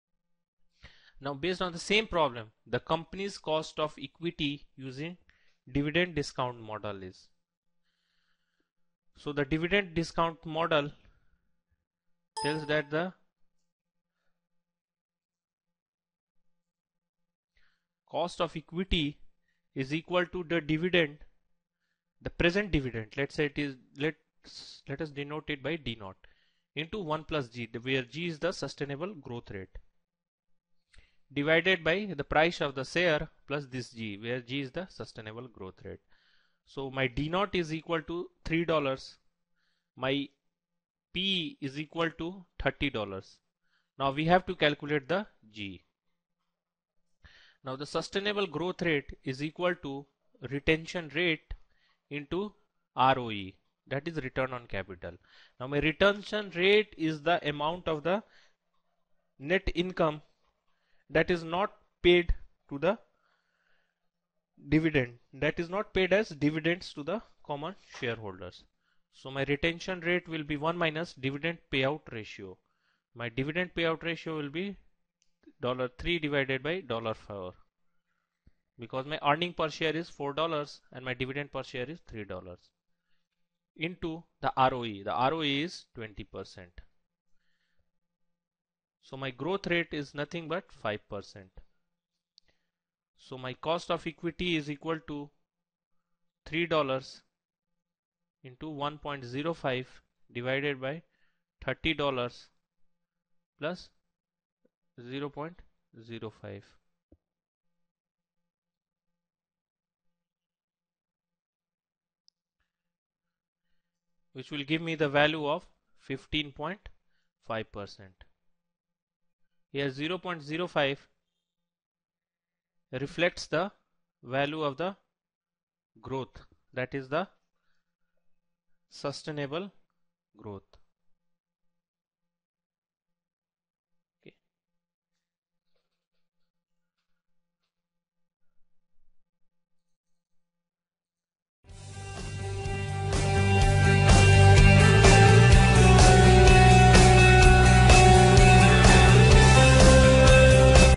CFA Tutorial: Corporate Finance (Calculating Cost of Equity using Dividend Discount Model)
Learn how to calculate cost of equity using dividend discount model.
Dividend Discount Model (DDM): It is a method of valuing a company based on the theory that a stock is worth the discounted sum of all of its future dividend payments.